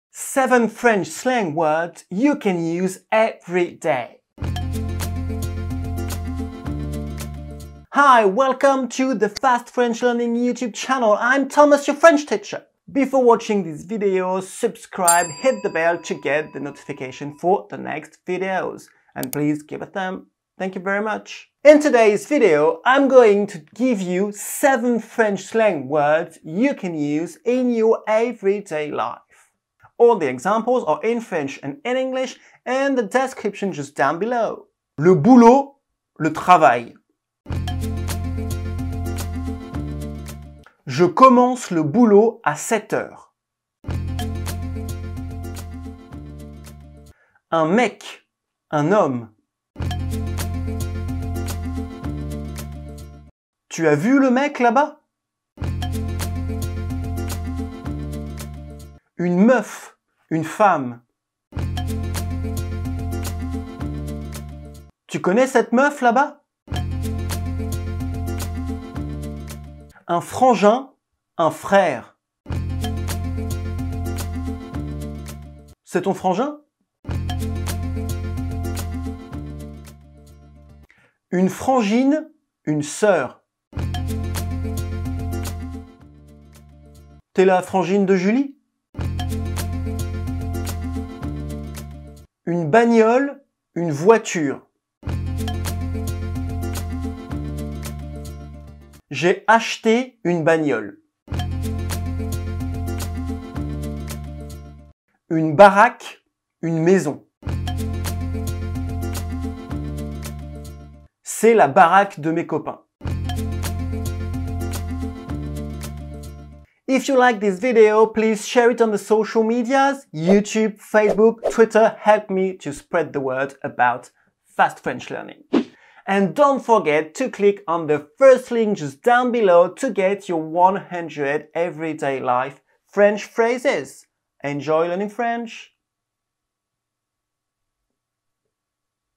7 FRENCH slang words
7 FRENCH slang words

Enjoy your learning French😉

Le boulot = le travail (the work)
Je commence le boulot à 7 heures
I start work at 7am

Un mec = un homme (a man)
Tu as vu le mec là-bas ?
Have you seen the guy over there?

Une meuf = une femme (a woman)
Tu connais cette meuf ?
Do you know this chick?

Un frangin = un frère (a brother)
C’est ton frangin ?
Is he your brother?

Une frangine = une soeur (a sister)
Je suis la frangine de Julie
I’m Julie’s sister

Une bagnole = une voiture (a car)
J’ai acheté une bagnole
I’ve bought a car

Une baraque = une maison (a house)
C’est la baraque de mes copains
This is my friends' house